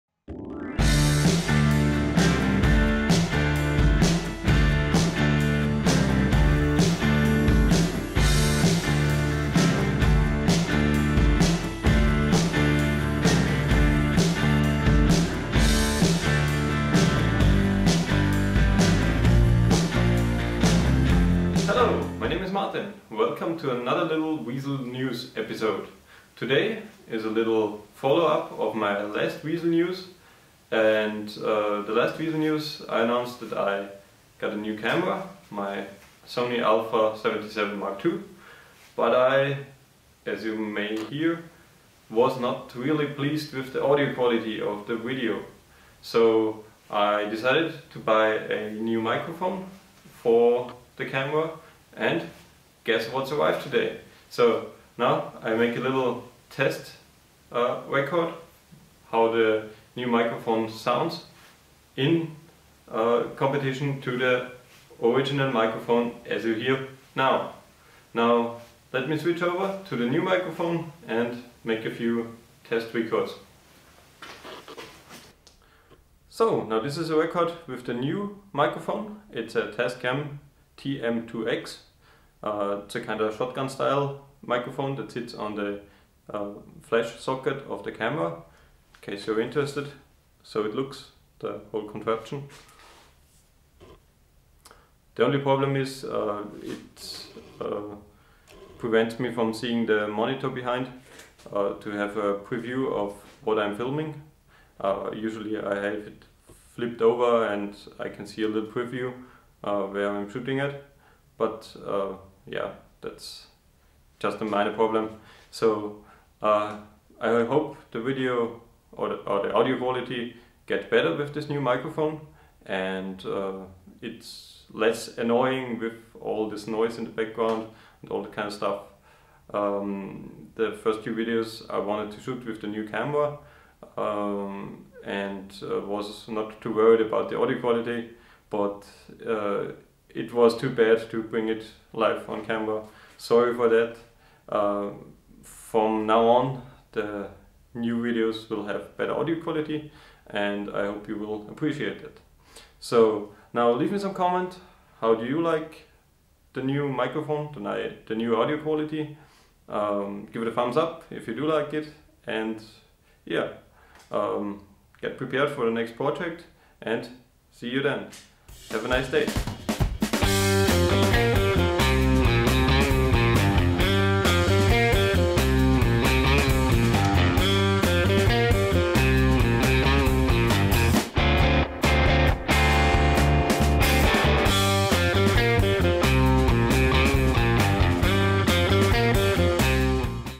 WeaselNews #6 New Microphone 🌳
Welcome to the new “WeaselNews” episode!
Today just a little audio test of my new microphone to increase the audio quality of my videos. Please leave me some comments how you like the better voice intelligibility.

The microphone is a “TASCAM TM-2X”

PS: I'm not a native english guy, apologies for the clumsy accent, don't be to harsh, I’ll get better I SWEAR! ;)

Greetings Martin
 and “May the weasel be with you!”